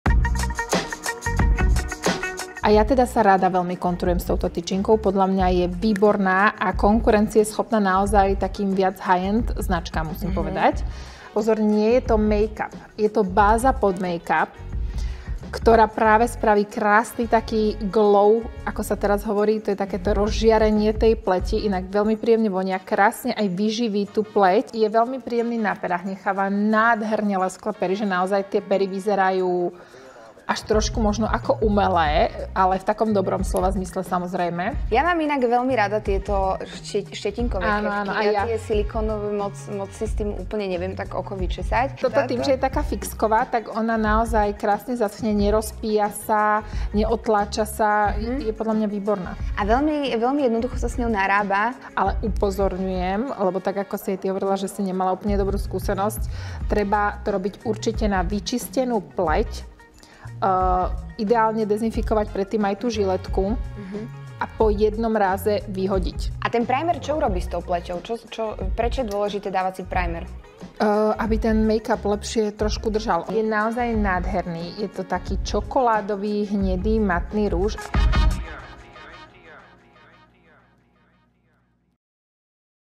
dmLIVE: Zoznámte sa s jesennými novinkami zn. Catrice
V našom poslednom dmLIVE streame sme sa venovali jesenným novinkám značky Catrice spolu s influencerkou Alexandrou Molnárovou alias @operenecholec a moderátorkou Jankou Slačkovou.